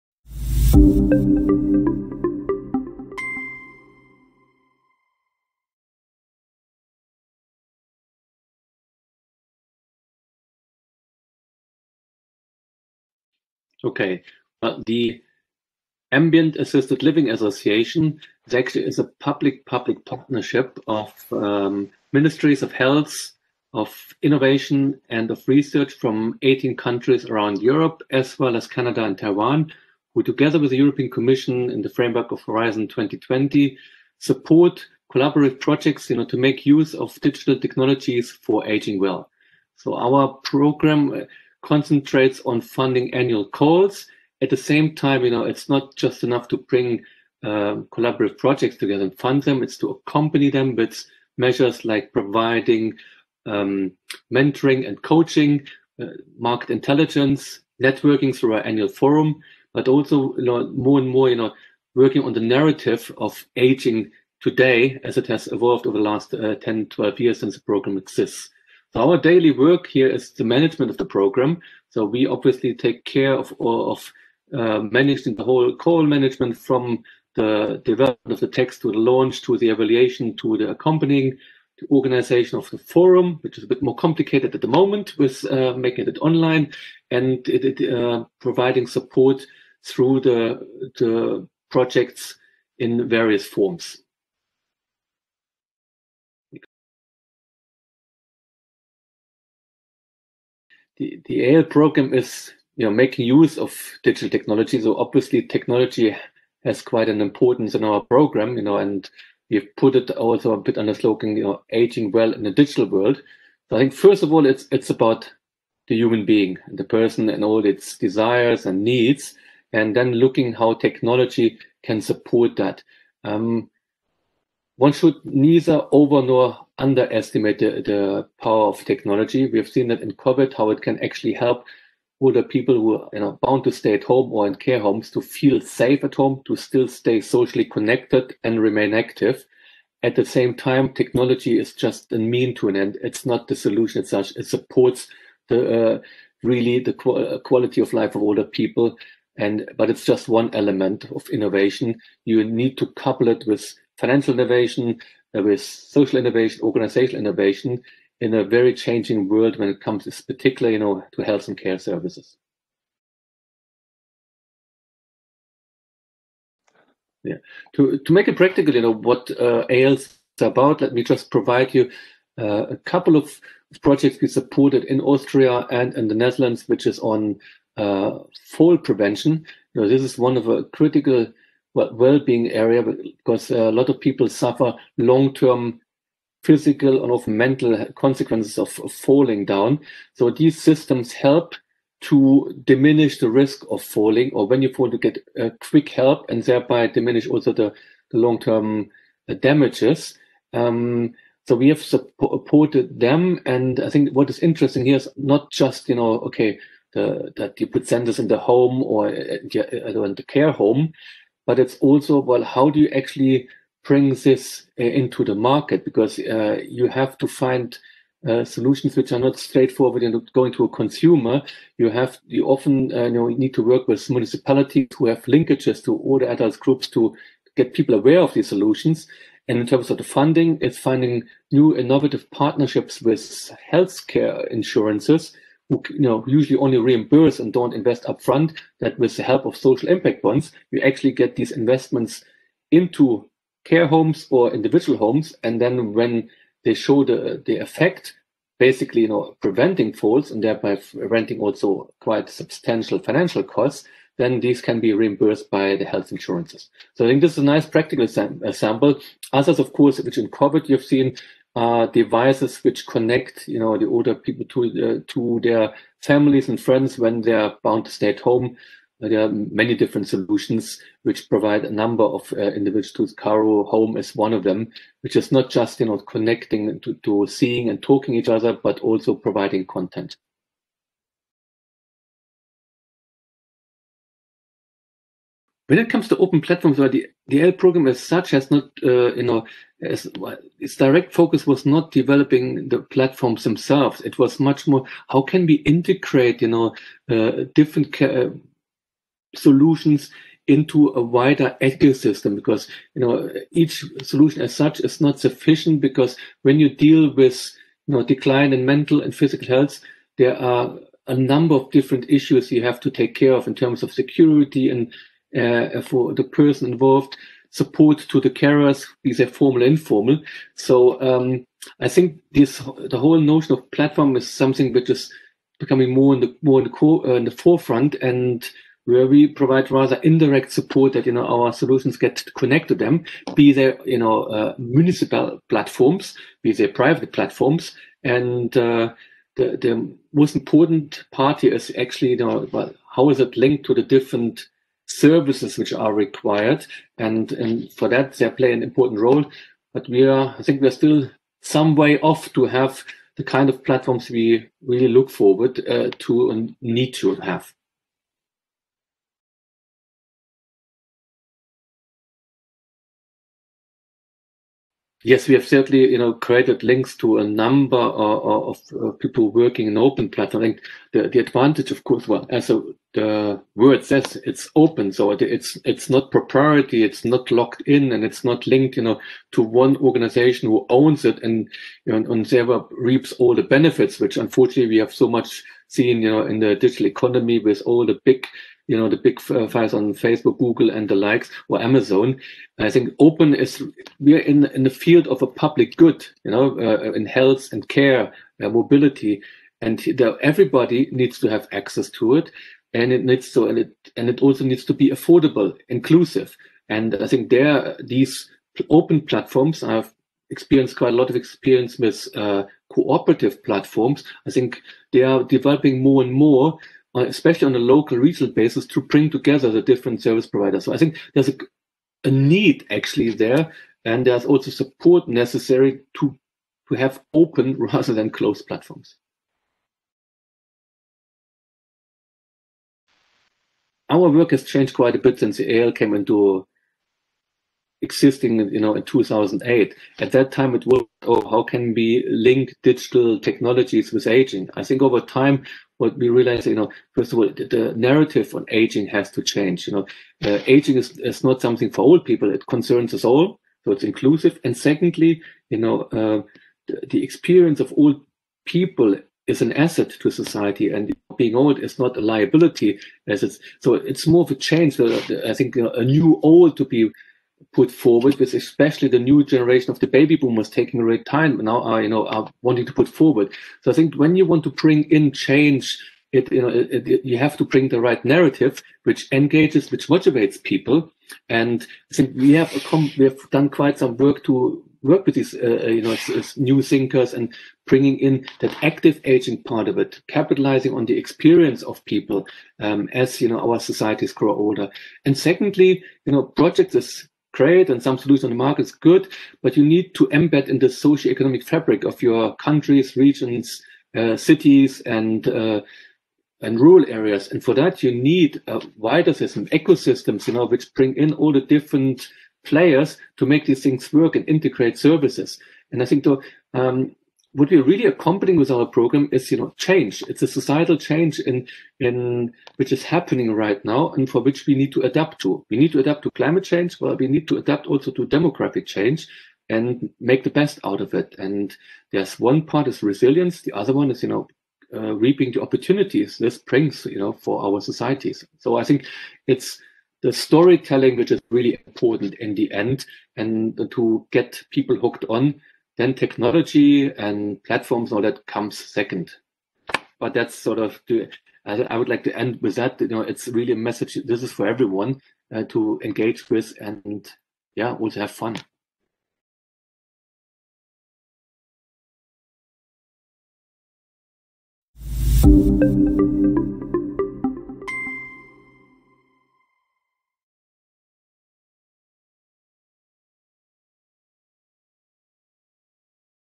AHA Experts' Voices: Klaus Niederländer on the role of technology in healthy ageing
Mr Klaus Niederländer is the Director of Central Management Unit of AAL Programme which funds applied research for developing ICT-based innovation for active and healthy ageing. In this interview Mr Niederländer talks on the role of technology in providing older people with better quality of life and improving the parameters in their daily living. In the interview he presents information on how innovation is actually applied in practice in the field of “fall-prevention” and how new technologies can bring financial benefits to their users.

Further, the interviewee highlights the importance of functional connectivity between open platforms and other devices and services in the field creating one big ecosystem for the benefit of older people. Mr Niederländer concludes with the remark that demographic change brings many challenges to our societies but also vast opportunities which are stored in today’s world of connected technology.